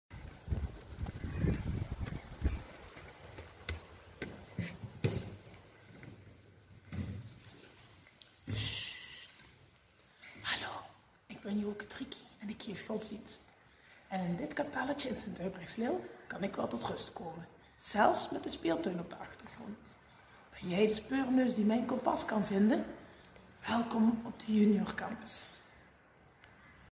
Kompaszoektocht 01
In de maand mei stelden heel wat leerkrachten een plekje voor dat aansluit bij hun interesses of vak(ken).
Ze hingen er toen een kompas op en daagden (toekomstige) leerlingen uit om dat te vinden. De plekjes kan je nog steeds (be)zoeken, het kompas is er intussen wellicht al verdwenen.
Hier vind je een selectie van wat we toen via Facebook lanceerden.